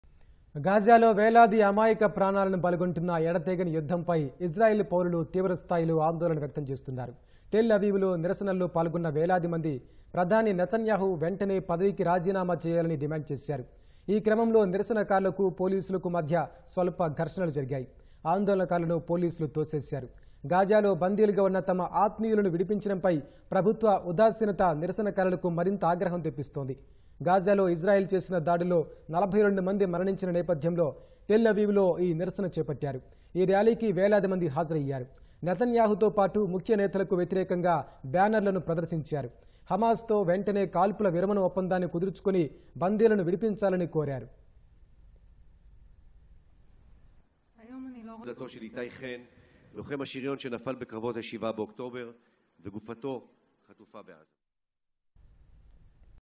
Israel People Anti-Govt Protests | Demands New Polls & Return of Hostages | ఇజ్రాయెల్ పౌరుల ఆందోళన
గాజాలో వేలాది అమాయక ప్రాణాలను బలిగొంటున్న ఎడతెగని యుద్ధంపై ఇజ్రాయెల్  పౌరులు తీవ్రస్థాయిలో ఆందోళన వ్యక్తం చేస్తున్నారు. టెల్ అవీవ్ లో నిరసనల్లో పాల్గొన్న వేలాదిమంది.. ప్రధాని నెతన్యాహు వెంటనే పదవికి రాజీనామా చేయాలని డిమాండ్  చేశారు. ఈ క్రమంలో నిరసనకారులకు, పోలీసులకు మధ్య స్వల్ప ఘర్షణలు జరిగాయి. ఆందోళనకారులను పోలీసులు తోసేశారు. గాజాలో బందీలుగా ఉన్న తమ ఆత్మీయులను విడిపించడంపై ప్రభుత్వ ఉదాసీనత నిరసనకారులకు మరింత ఆగ్రహం తెప్పిస్తోంది.
గాజాలో ఇజ్రాయెల్ చేసిన దాడుల్లో 42 మంది మరణించిన నేపథ్యంలో టెల్  అవీవ్ లో ఈ నిరసన చేపట్టారు. ఈ ర్యాలీకి వేలాదిమంది హాజరయ్యారు. నెతన్యాహుతో పాటు ముఖ్య నేతలకు వ్యతిరేకంగా బ్యానర్లను ప్రదర్శించారు. హమాస్ తో వెంటనే కాల్పుల విరమణ ఒప్పందాన్ని కుదుర్చుకుని బంధీలను విడిపించాలని కోరారు.